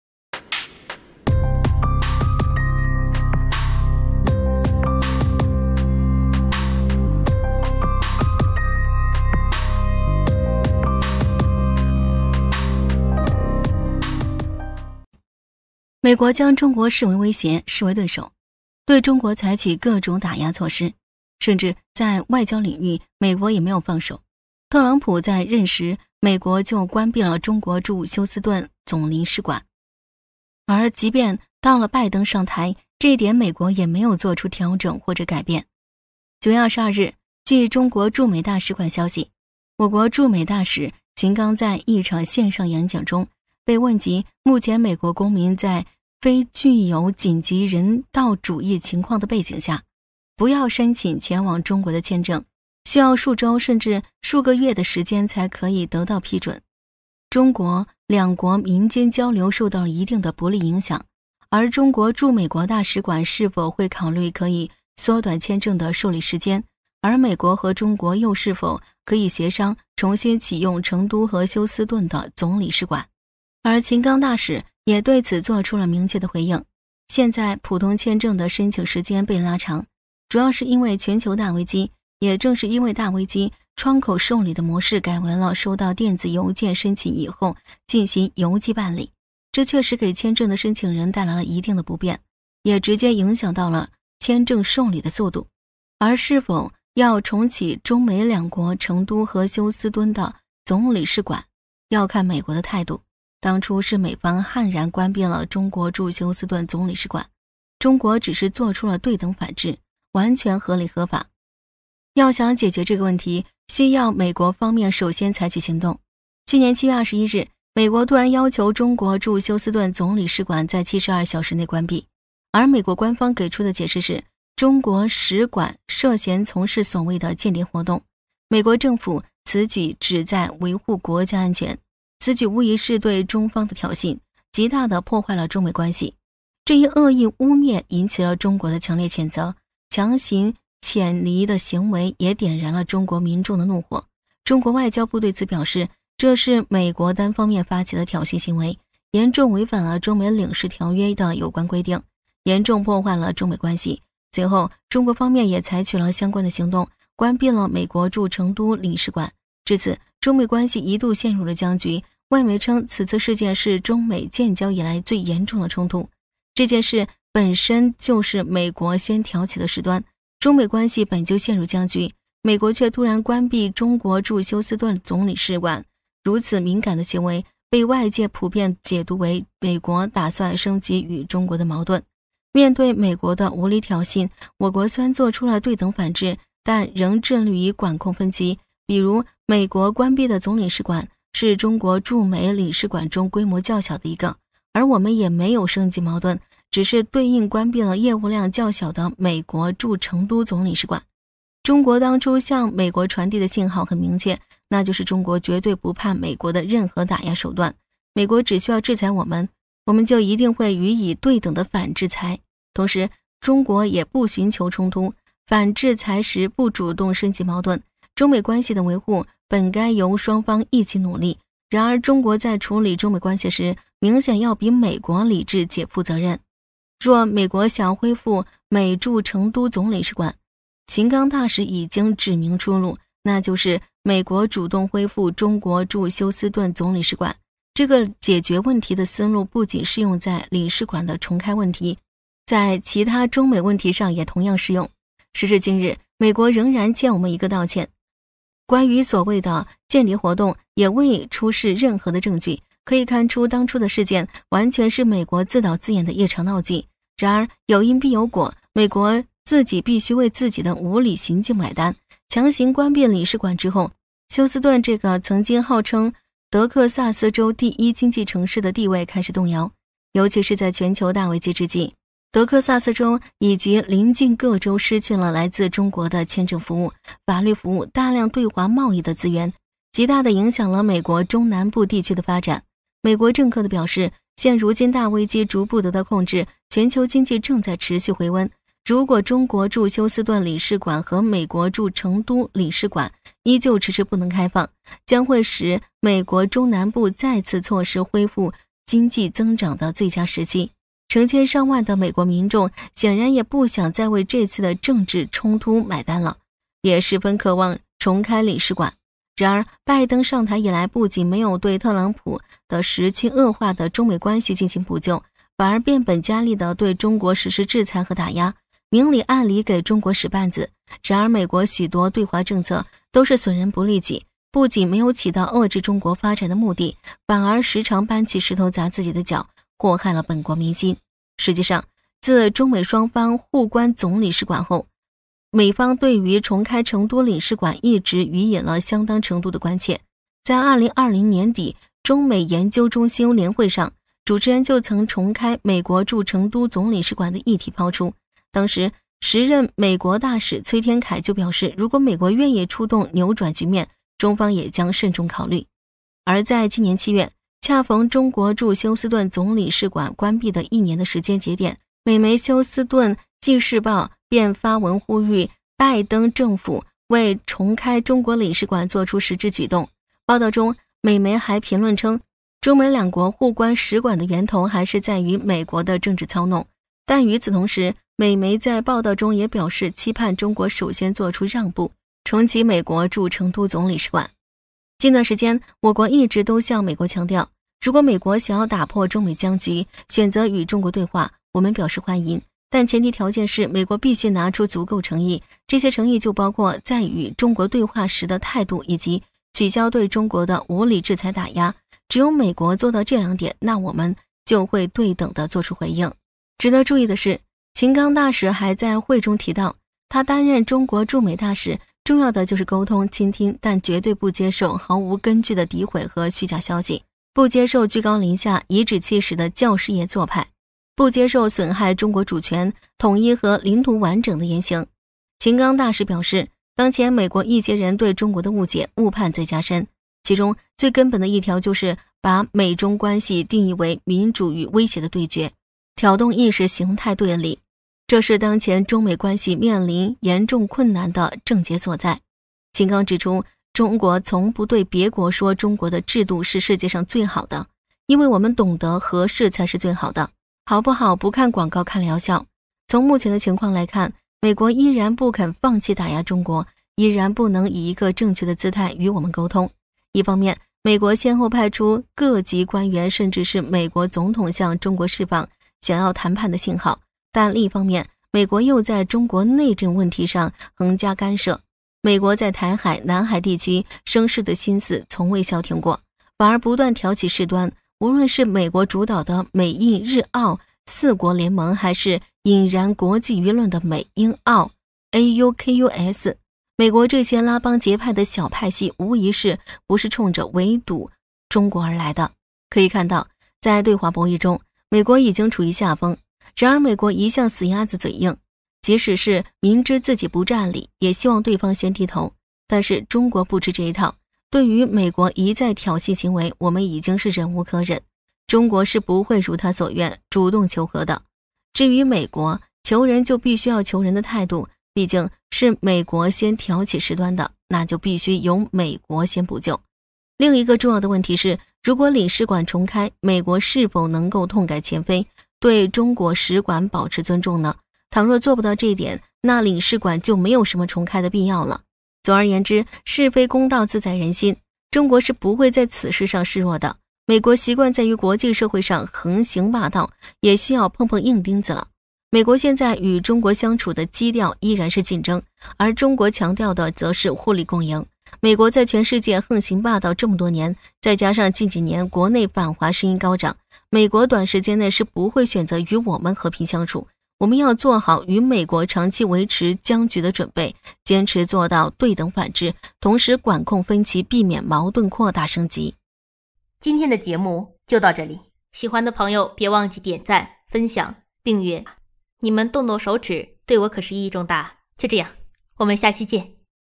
关闭14个月后，美国想要重开成都总领事馆，秦刚大使把话挑明，一句话震动世界，美国从未如此难堪
每天分享最新的热门新闻。
祝大家每天都有一个好心情。